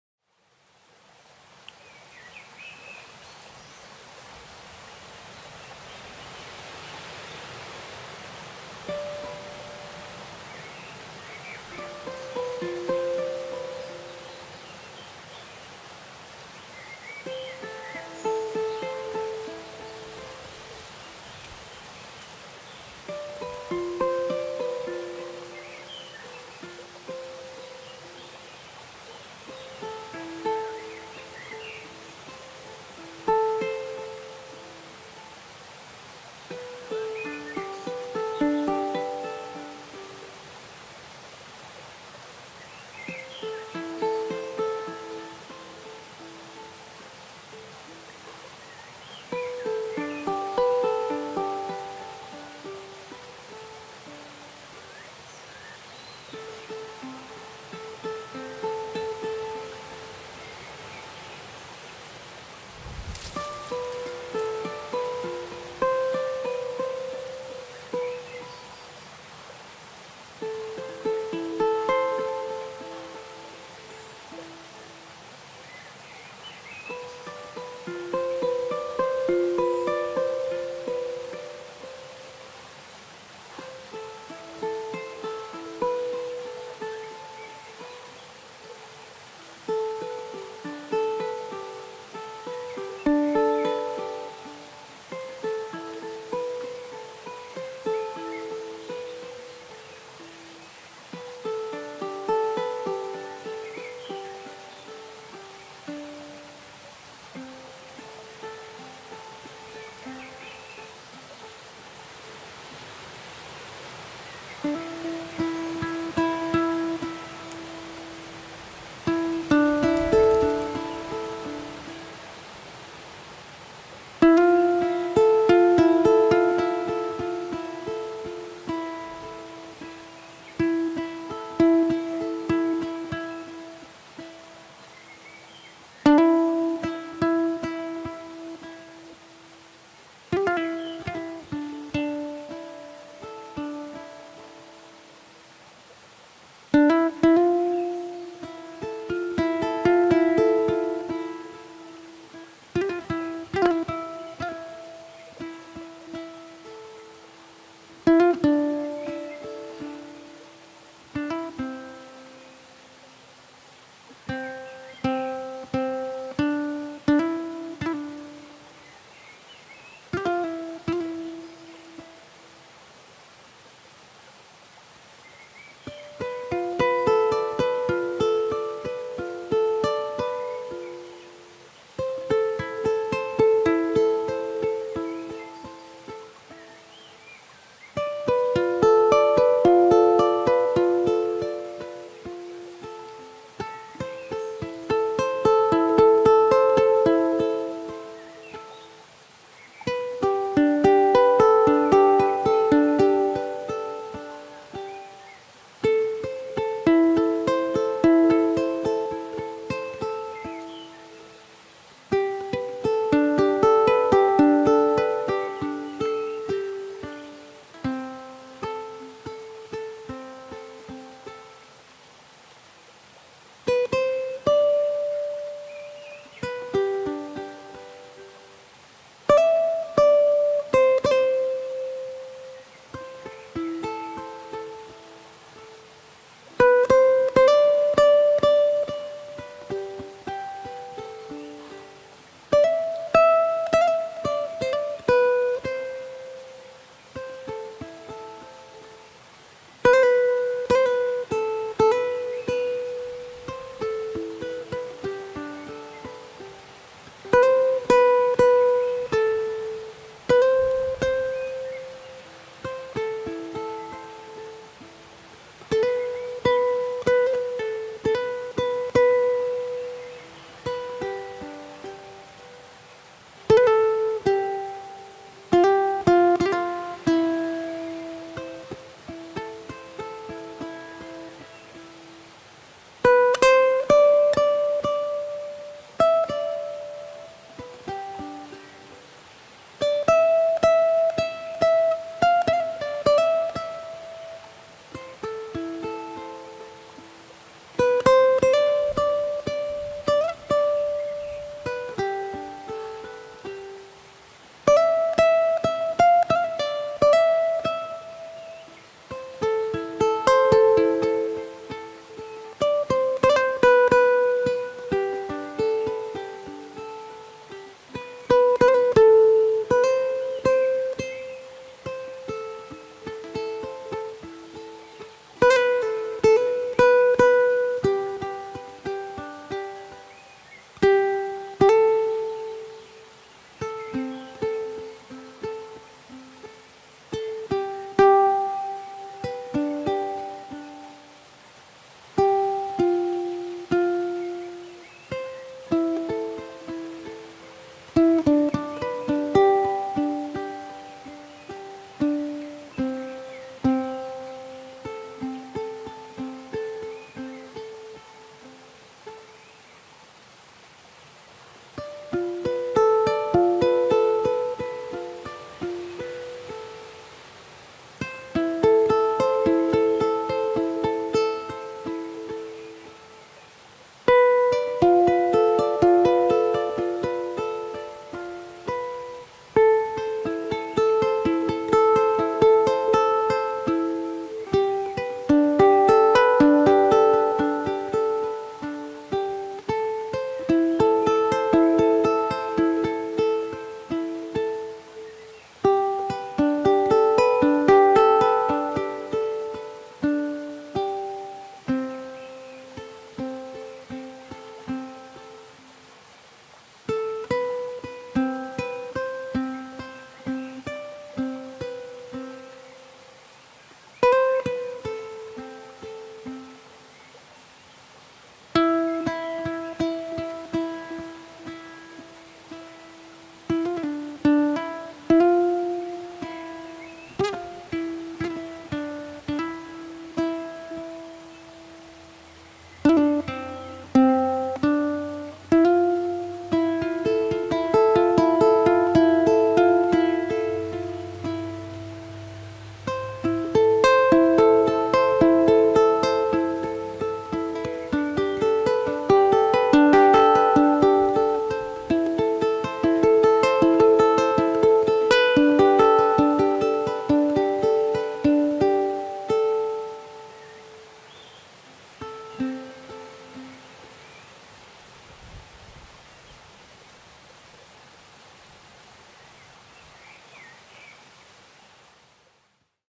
Relaxing Ukulele Song - Relaxing Calm Stress Relief Music - Nature sounds , Sleep, Meditation
Relaxing Ukulele Song - Relaxing Calm Stress Relief Music - Nature sounds , Meditation, Sleep

Hello my friends hope you are well !
Some days ago i was for the weekend on the village at the mountains.
I was not ready for recording but the weather was nice and not much people around ( actually no one was around).
So i decide to try record a relaxing melody on my friends Ukulele (i have to buy one sometime).
Actually there was some audio recording problems,it is not so easy to make everything by your self but i believe the final work is ok! :)
Here it is my last improvisation in nature.
Sweet warm soft ukulele music, use it to calm relax practising yoga or meditation.

You can see that playlist if you like with all my improvisation in nature.

Calm and feel Relax hearing smooth relaxing music and dream watching those beautiful landscapes and nature miracles on your monitor or tv.

Be well and happy ... Summer is coming ... Keep on dreaming...

or

for more info about my work.